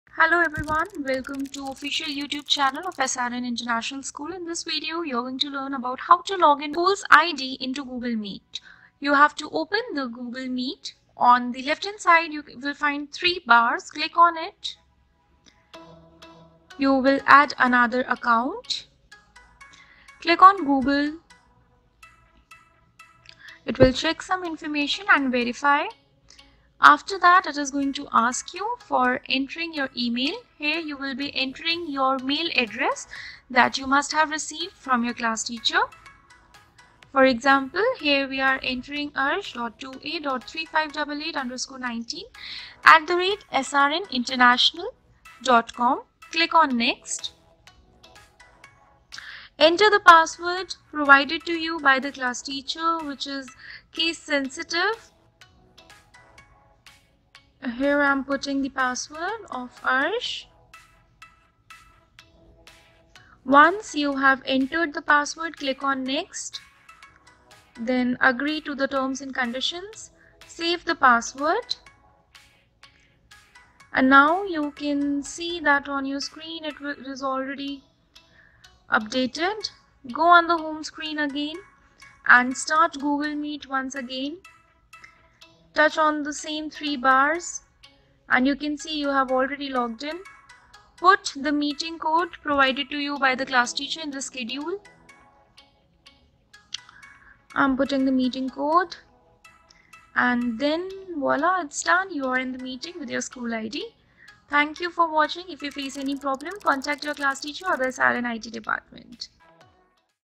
How to use SRN Gmail ID for Google Meet l Online Classes I Assesments I Mobile Access I SRN IT Team
Here is the video how can you use this id  to login into Google Meet. Watch the video.